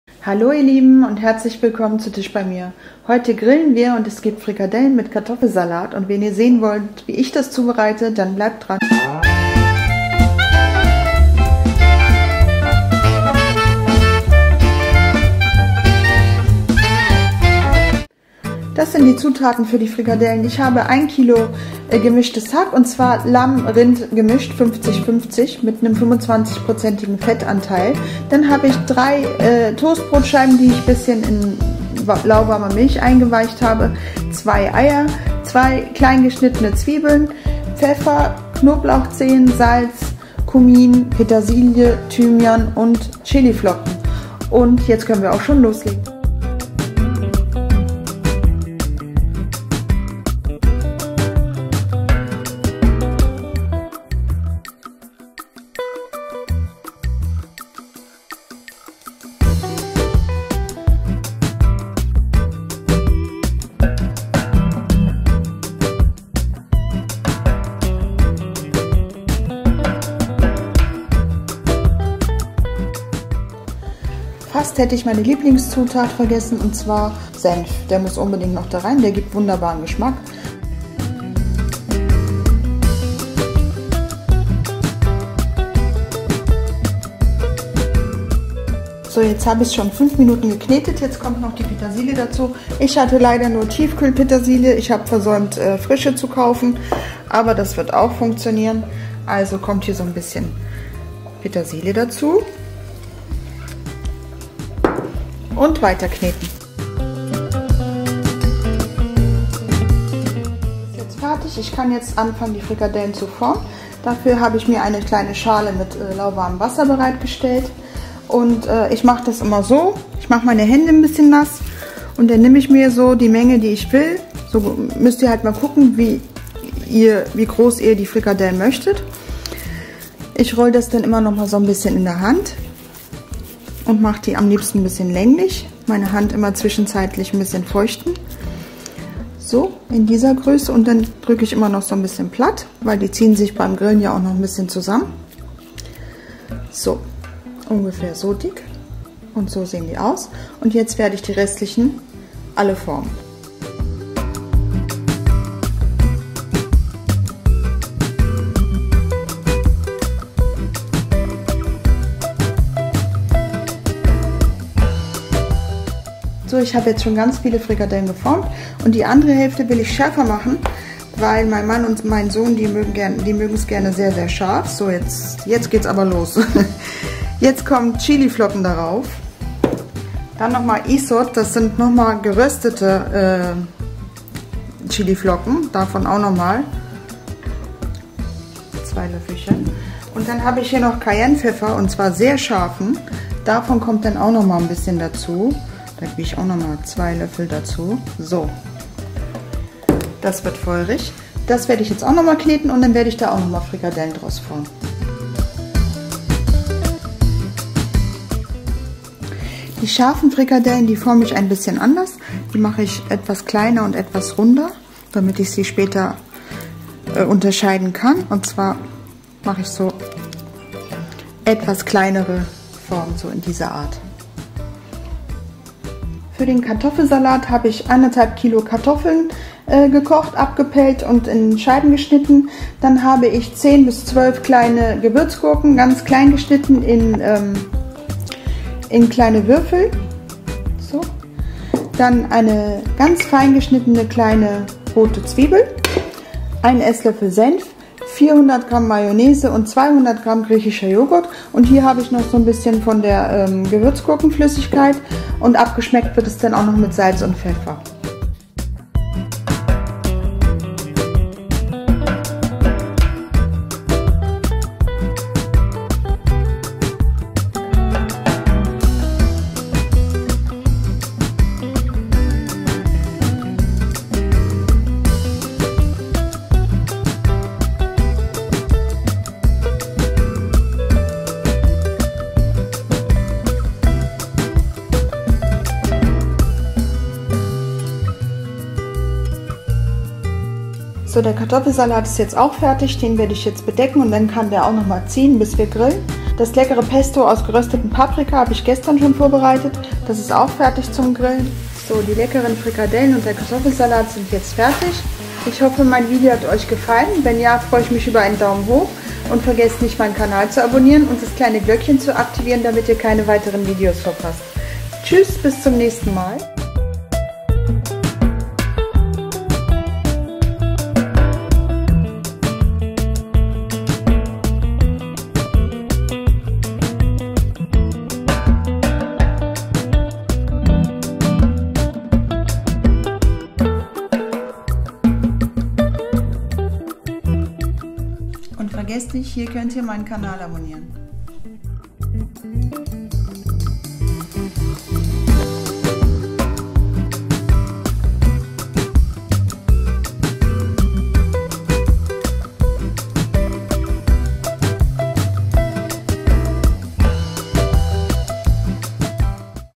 Grillen auf Balkonien⁉️🌞Leckere Frikadellen und schneller Kartoffelsalat ✅
Heute wollen wir grillen deswegen muss der schnelle Kartoffelsalat und die leckeren Frikadellen her. Nix Besonderes. Alles schnell gemacht. Kann ich nur empfehlen.
Probiert's unbedingt mal aus!
Hier sind die Zutaten für das Rezept:
Zutaten für die Frikadellen:
●1kg gemischtes Hackfleisch (Lamm-Rind)
●2 Zwiebeln, sehr klein geschnitten
●2 Eier
●3 Scheiben Toastbrot in 100ml lauwarmer Milch eingeweicht
●1 EL Senf
●2 Koblauchzehen
●1 1/2 TL Cumin
●1 1/2 TL Pfeffer
●1 TL Chiliflocken
●1 TL Thymian
●1 -2 EL gehackte Petersilie
●Salz
Zutaten für den Kartoffelsalat:
✺1 1/2 kg gekochte Kartoffeln
✺1 kleine rote Zwiebel, ganz klein geschnitten
✺400g Majonaise
✺200g griechischer Joghurt
✺1 EL Senf
✺10-12 kleine geschnitten Cornichons /Gewürzgurken
✺60ml Gurkenwasser
✺Salz/Pfeffer
Schwarzes Emailleblech*
Ähnlicher Messbecher*
Leonardo Glasschüssel eckig*

Filiz